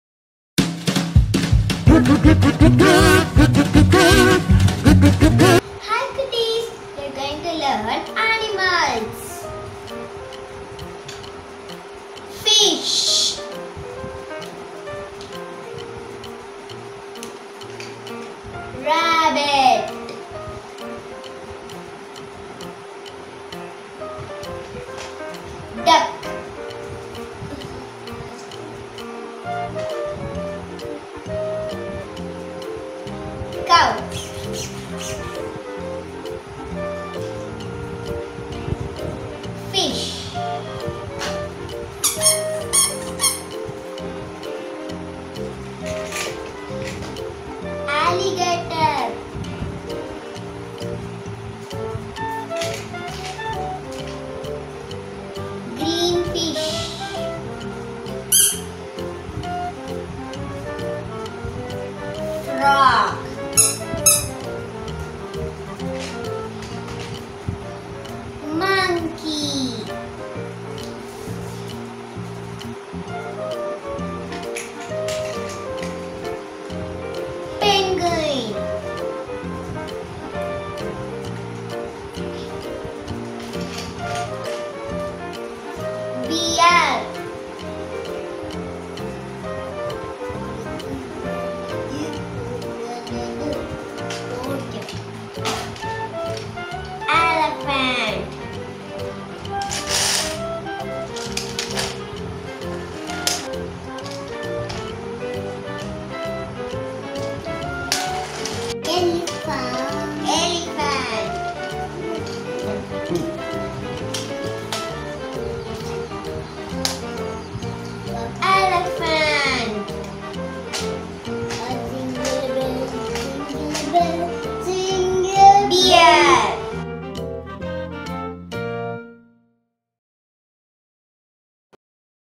Animals Name |Zoo Animals| Kids Toys | Sea animals | Farm Animlas | Learn animals name| CIC TV | P1
We are going to show you some toy animals. This is a fun and an educational video for kids where children will learn some animal names.